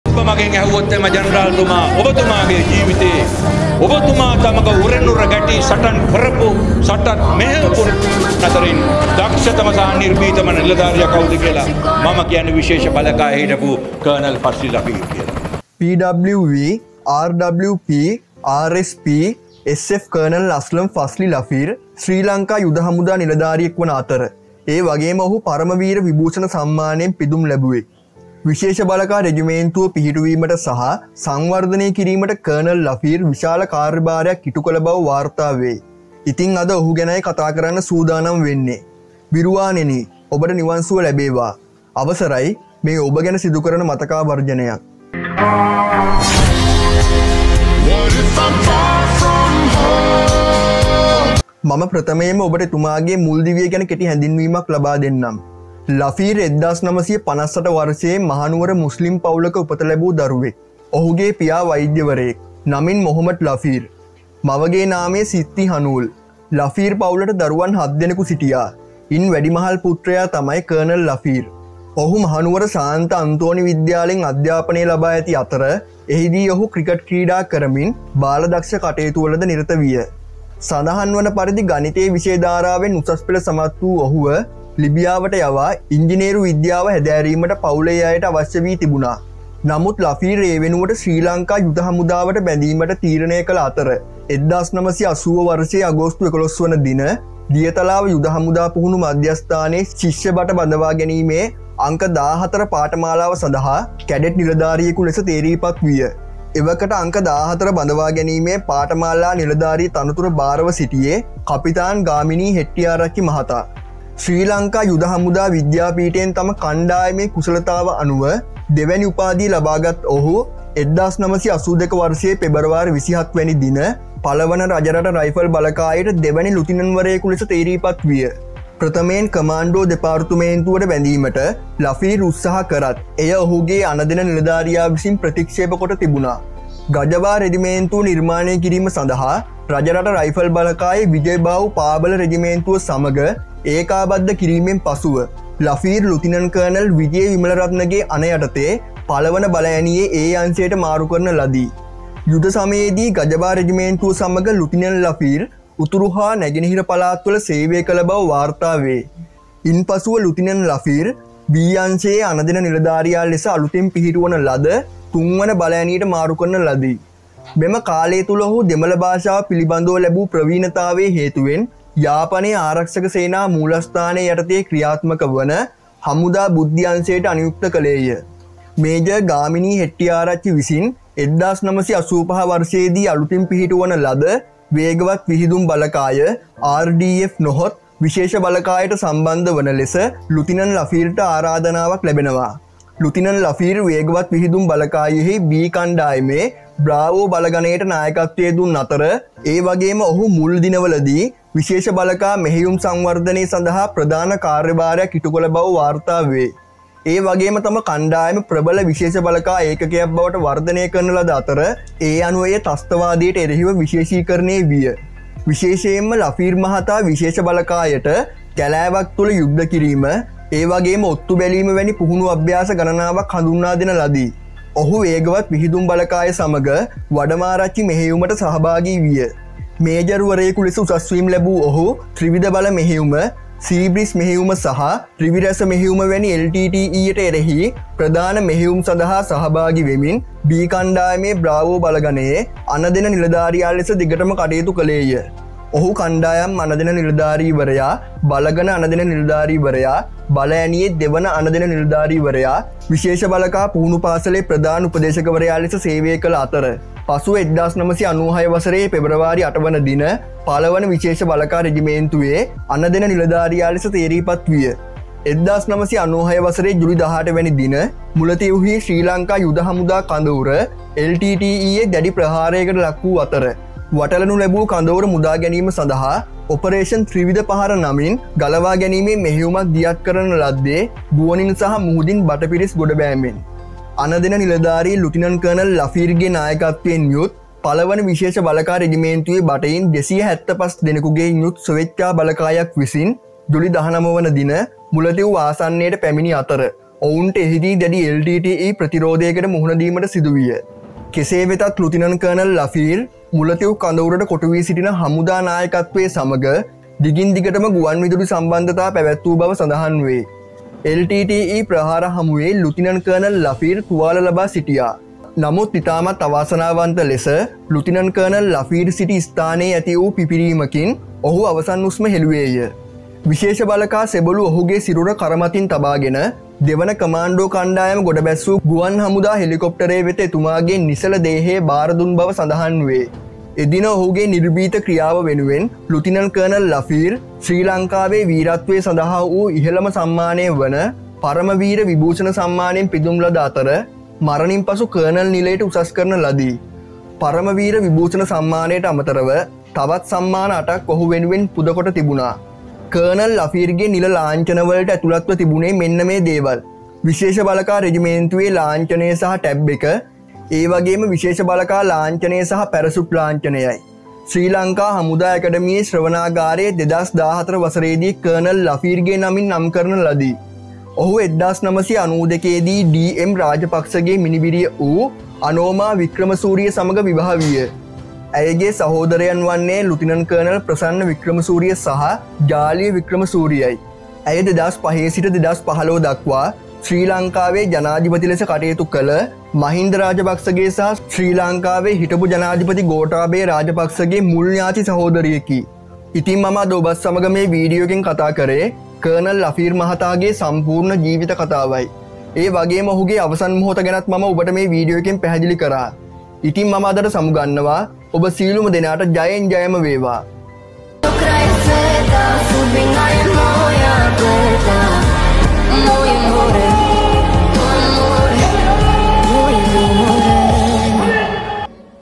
කර්නල් ෆස්ලි ලෆීර්ගේ අවසන් මොහොත | Colonel Fazli Lafir : Sri Lanka Army
😥 ලෆීර් 1958 වර්ෂයේ මහනුවර මුස්ලිම් පවුලක උපත ලැබූ දරුවෙක්. ඔහුගේ පියා වෛද්‍යවරයෙක්. නමින් මොහොමඩ් ලෆීර්. මවගේ නාමය සිත්ති හනුල්. ලෆීර් පවුලට දරුවන් හත් දෙනෙකු සිටියා. ඉන් වැඩිමහල් පුත්‍රයා තමයි කර්නල් ලෆීර්.

කර්නල් ෆස්ලි ලෆීර්ගේ අවසන් මොහොත | Last moments of Colonel Fazli Lafir : Sri Lanka Army